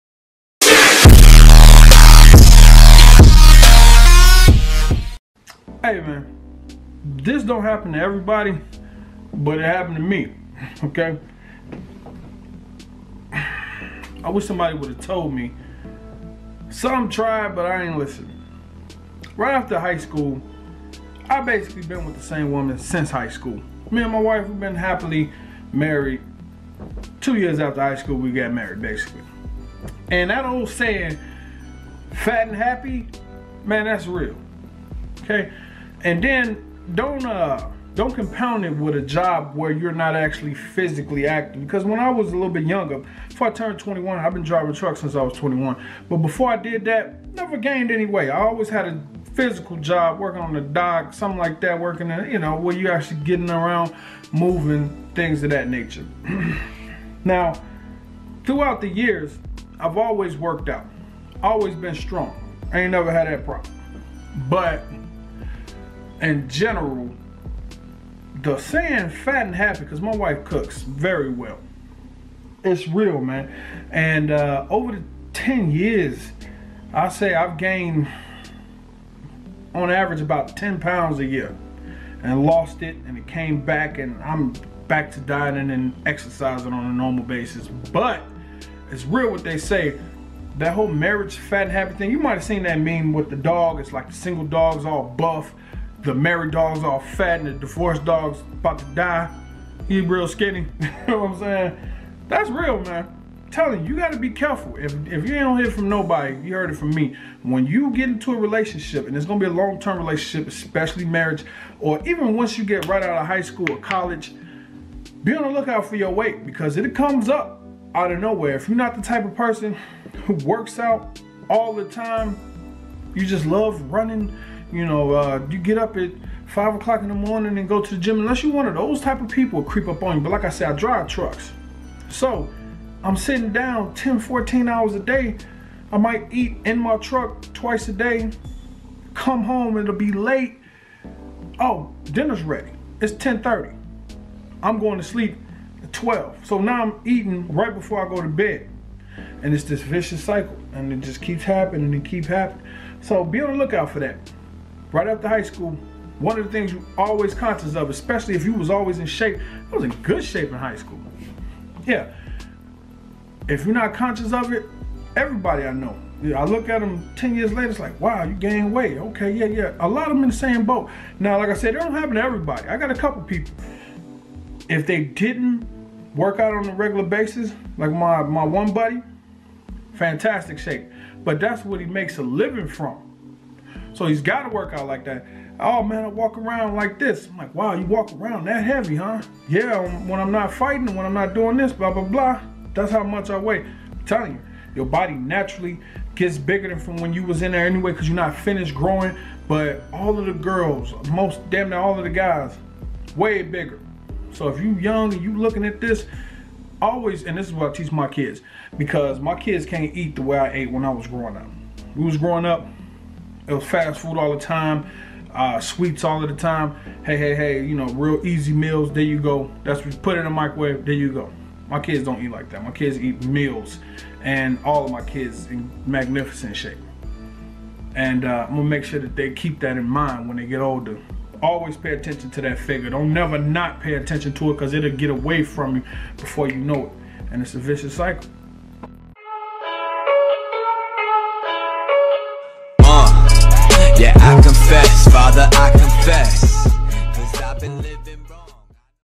Weight Gain after Highschool, College or Marriage
Just a few reasons why we tend to gain weight in certain times of our lives